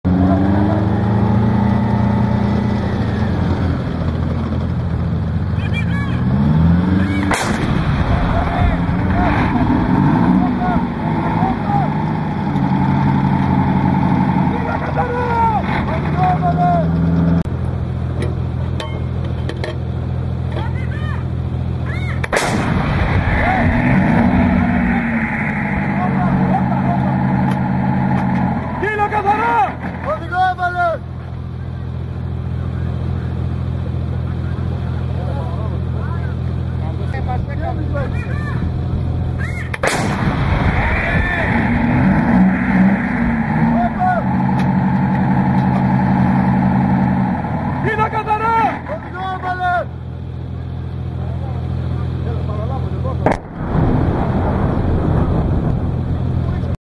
M110A2 SP firing.
Hellenic Artillery SP M110A2 firing three 203mm rounds in military exercise. Visible path and explosion of second and third round. Filmed with a Casio Ex F1 digital camera in high definition.